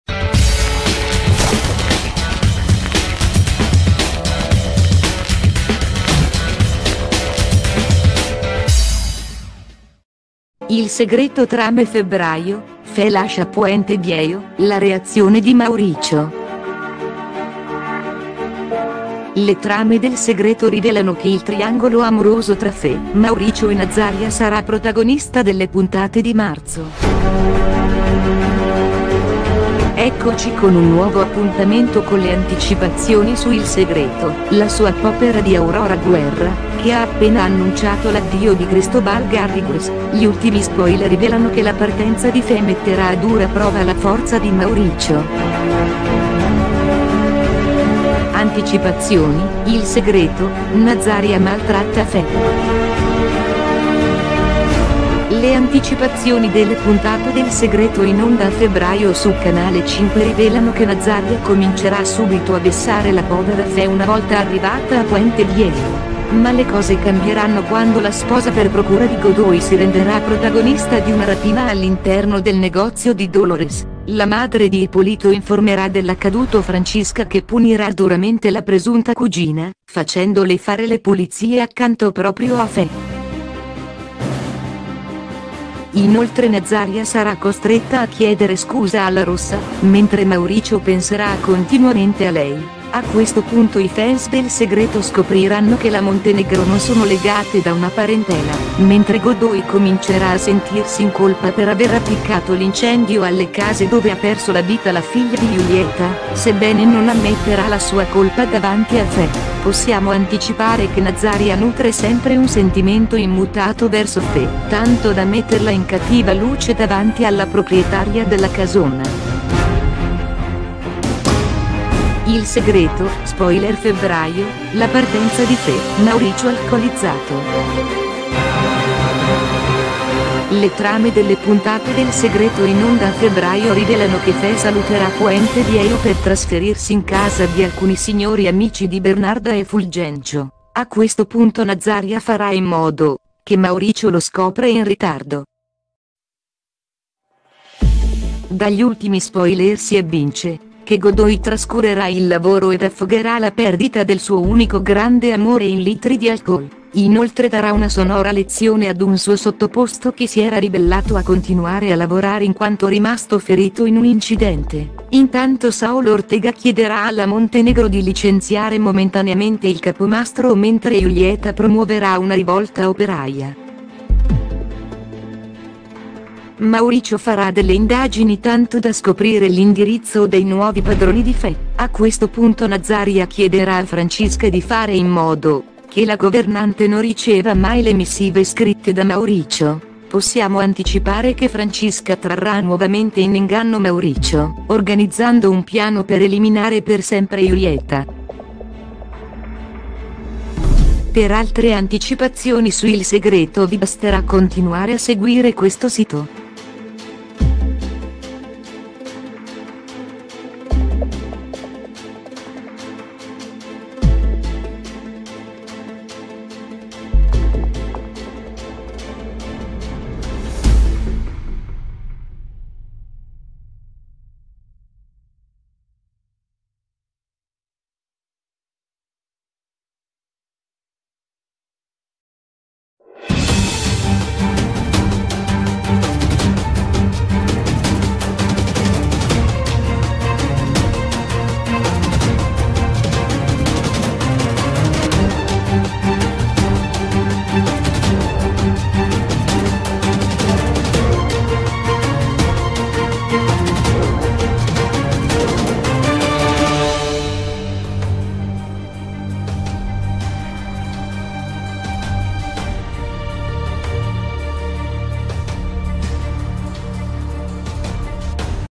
Il Segreto trame febbraio: Fè lascia Puente Viejo, la reazione di Mauricio
Le trame de Il Segreto rivelano che il triangolo amoroso tra Fè, Mauricio e Nazaria sarà protagonista delle puntate di marzo.